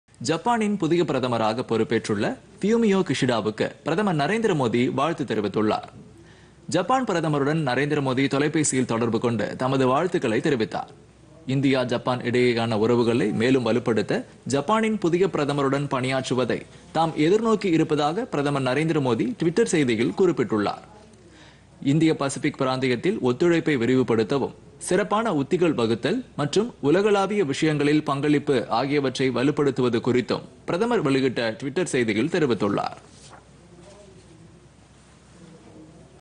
இந்தியா - ஜப்பான் நல்லுறவு வலுப்படும்PodhigaiTamilNews
இந்தியா - ஜப்பான்  இடையேயான நல்லுறவு மேலும் வலுப்படும் --  ஜப்பான் பிரதமருக்கு தொலைபேசியில் வாழ்த்து தெரிவித்த பிரதமர் நரேந்திர மோதி உறுதி.